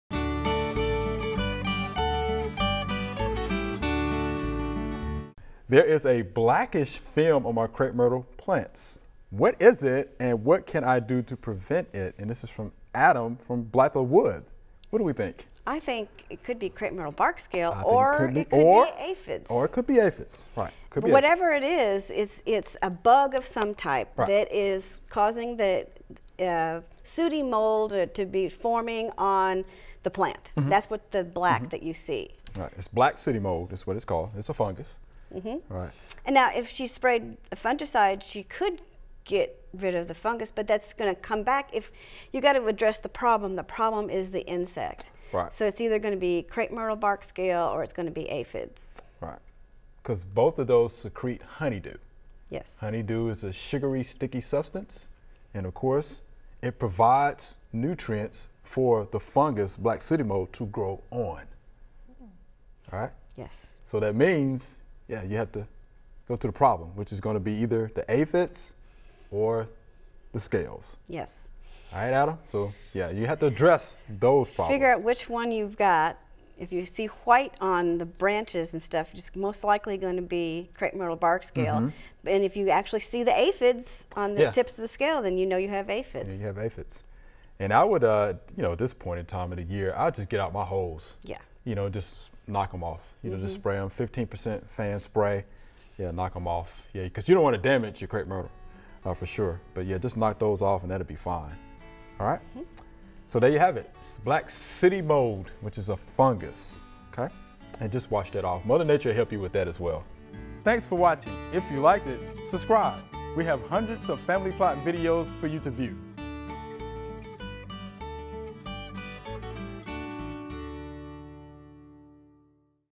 Q&A - What is the black film on my crape myrtle plants?
That is the fungus sooty mold. It is feeding on honeydew which is secreted by crape myrtle bark scale or aphids. If you don’t get rid of the scale or aphids the sooty mold will return.